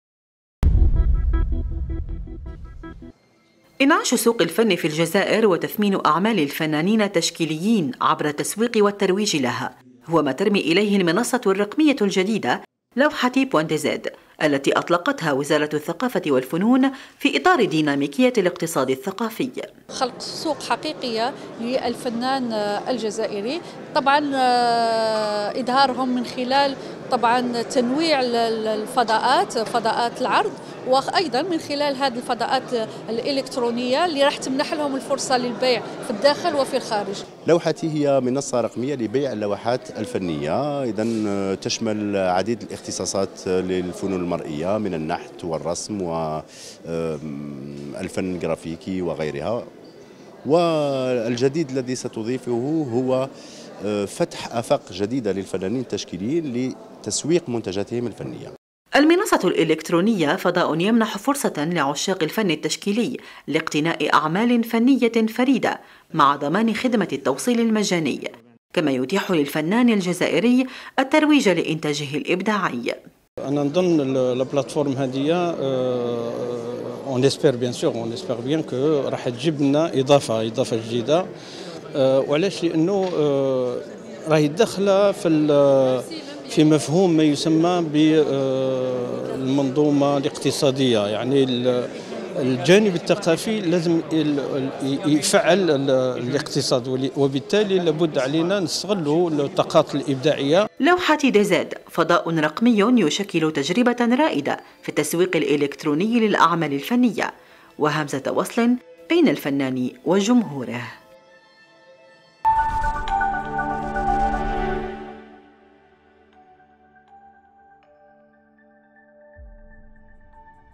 إطلاق المنصة الرقمية لوحتي لبيع الاعمال الفنية
الجزائر  (وأج)- اشرفت وزيرة الثقافة و الفنون مليكة بن دودة بالجزائر العاصمة على عملية إطلاق منصة
الكترونية "lawhati.dz" مخصصة لعرض و بيع الاعمال الفنية .